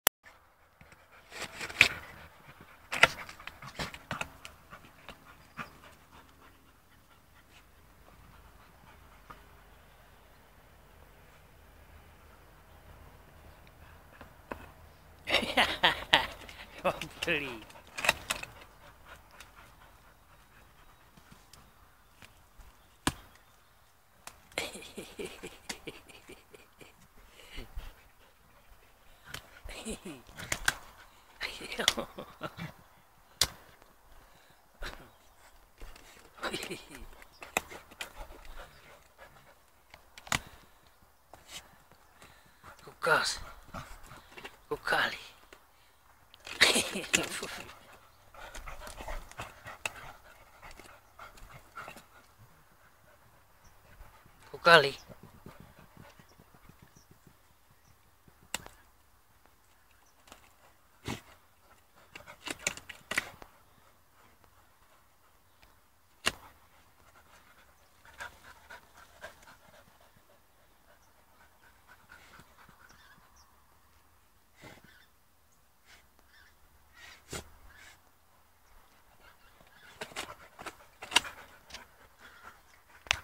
Mint n Liath Diggin
Two labs i hunted for ten years , mother and daughter . This is second day in of them uncovering the courigated iorn sheet on there own they would be normally doing this on earth only moving grown faster than a shovel hence them testing it with the mouths  . They found them the next day , look at there drive and determination ! And this is tame for them !